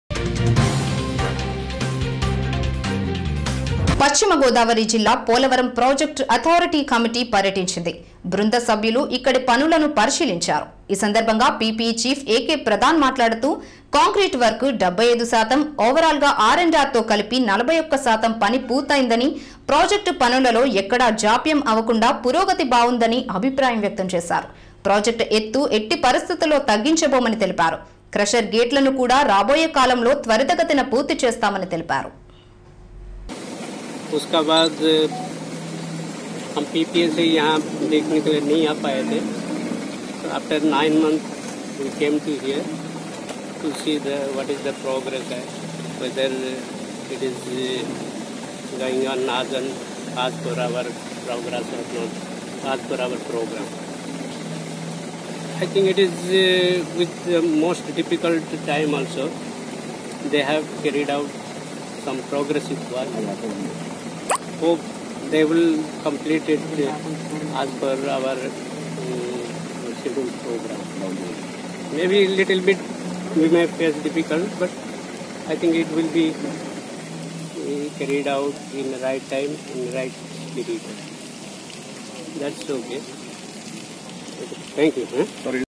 పశ్చిమ గోదావరి జిల్లా పోలవరం ప్రాజెక్టులో అథారిటీ కమిటీ పర్యటించింది.బృంద సభ్యులు || MY TV NEWS
పగో పోలవరం

పశ్చిమ గోదావరి జిల్లా పోలవరం ప్రాజెక్టులో అథారిటీ కమిటీ పర్యటించింది.బృంద సభ్యులు ఇక్కడి
పనులను పరిశీలించారు.ఈ సందర్భంగా PPE చీఫ్ AK ప్రధాన్ మాట్లాడుతూ కాంక్రీట్ వర్క్ 75శాతం, ఓవరాల్ గా ఆర్ అండ్ ఆర్ తో కలపి 41శాతం  పని పూర్తి అయిందని ప్రొజెక్ట్ పనులలో ఎక్కడా జాప్యం అవకుండా పురోగతి బాగుందని అభిప్రాయం వ్యక్తం చేశారు.
ప్రొజెక్ట్ ఎత్తు ఎట్టిపరిస్థితుల్లోనూ తగ్గించబోమని తెలిపారు.
క్రషర్ గేట్లును కూడా రాభోయే కాలంలో త్వరతగతిన పూర్తి చేస్తామని తెలిపారు.